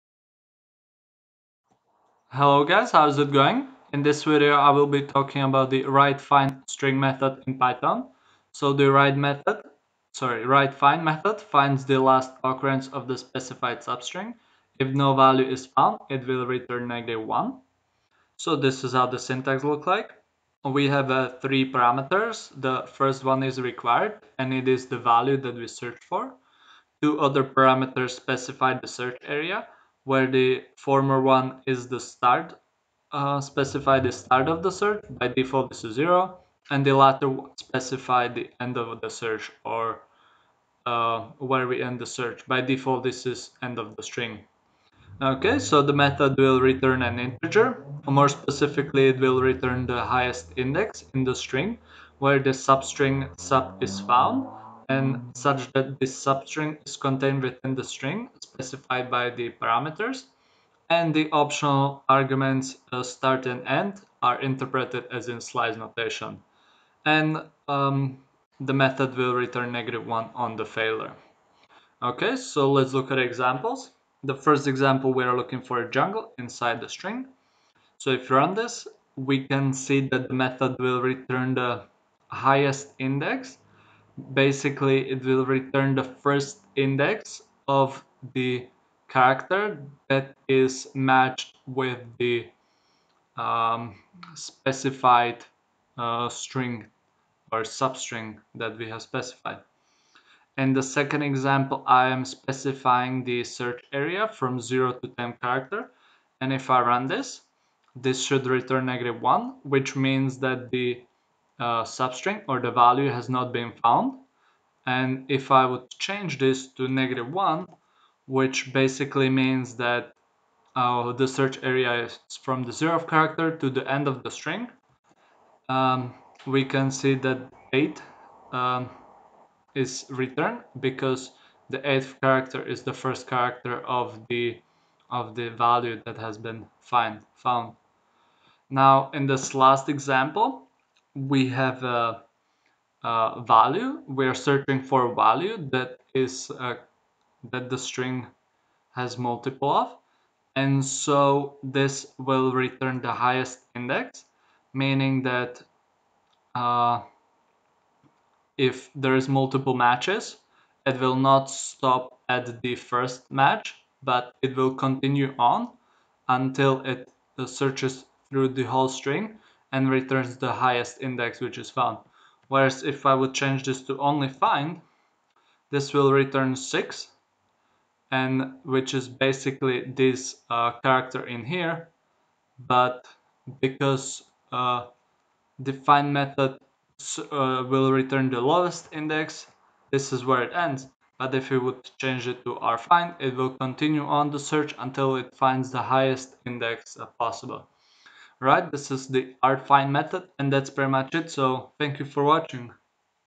Python rfind() string method
This is just a quick reference guide going over the python build-in methods. In this video, I am showcasing the string .rfind() method along with some of the examples and common use cases